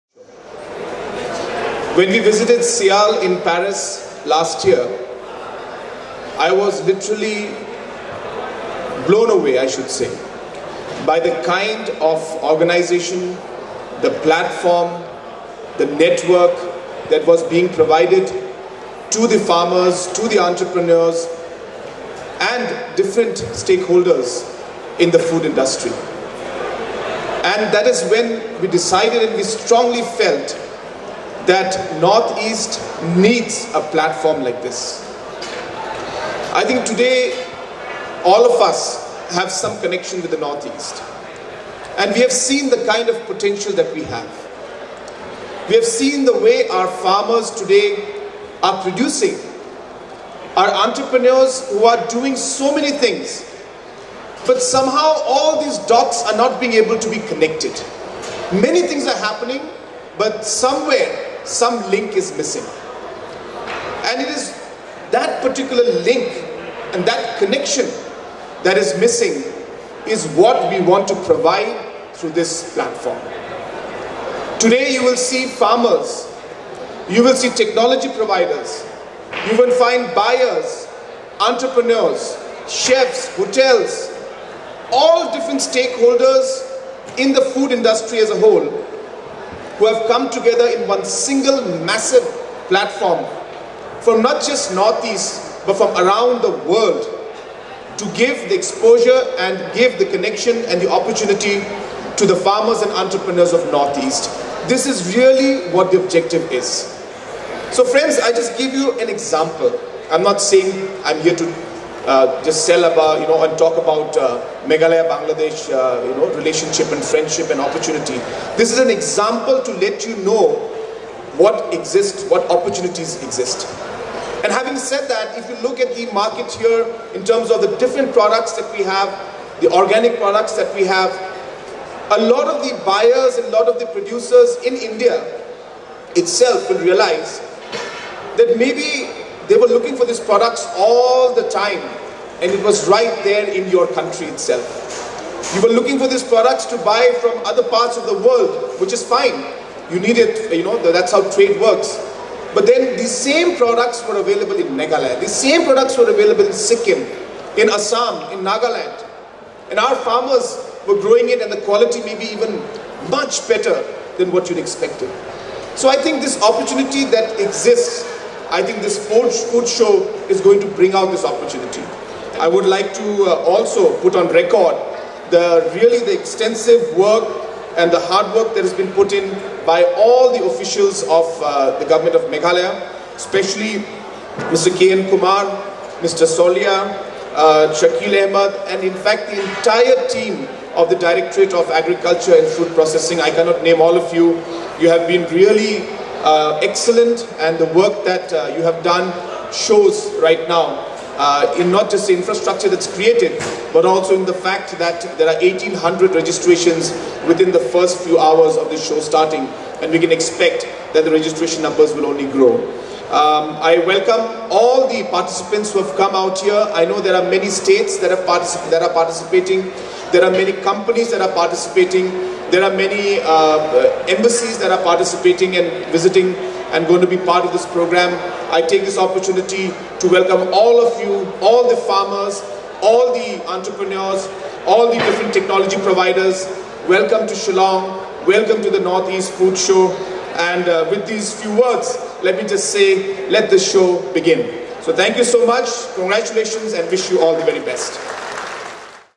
Conrad K Sangma | Chief Minister's Conclave | NEFS
The Chief Minister’s Conclave at the NortheastFoodShow in Shillong gave an opportunity for CMs from three Northeastern States- Arunachal, Tripura and Sikkim to come together to pledge for a common platform to promote the diverse potential of our Agri-Horti and allied sectors.